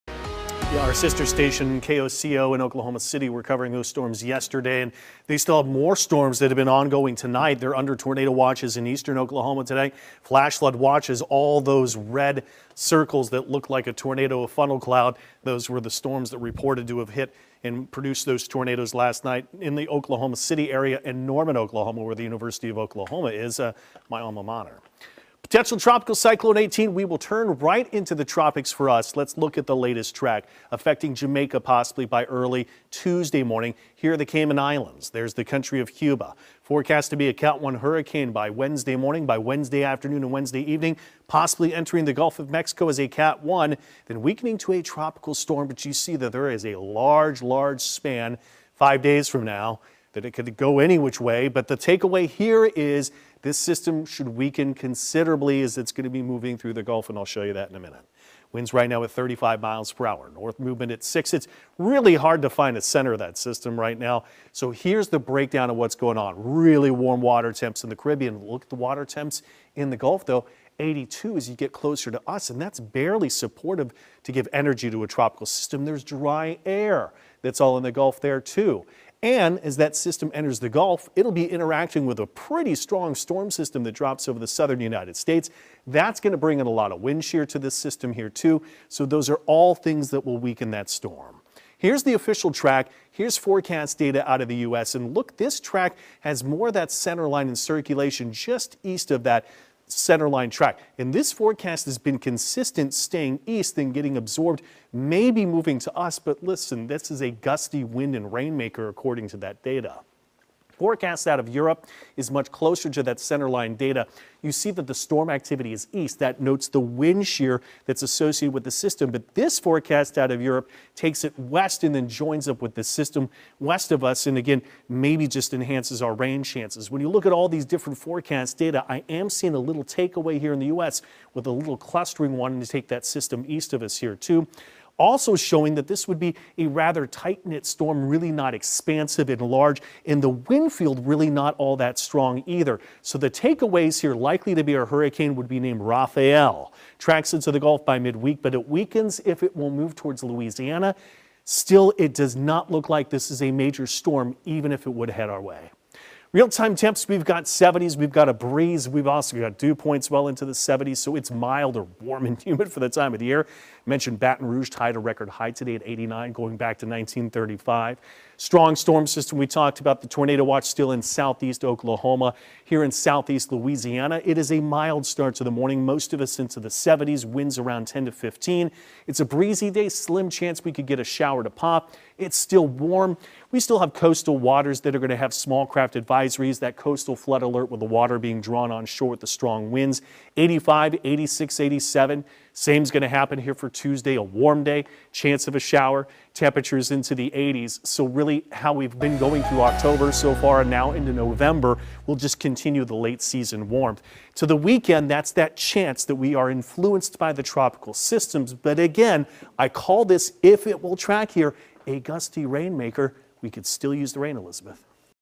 A tropical forecast and monitoring our local weather too
Meteorologist Devon Lucie starts us off in the Caribbean tracking what's expected to be Hurricane Rafael and could enter the Gulf of Mexico by the middle of the week letting you know what's the likeliest track and what impact it could have on Louisiana, then turns to our local weather over the next two days highlighting what our temperatures will be, rain chances, how strong the winds will be and how that will affect marine conditions, then finishes with the 7 day forecast wrapping all the information up for you.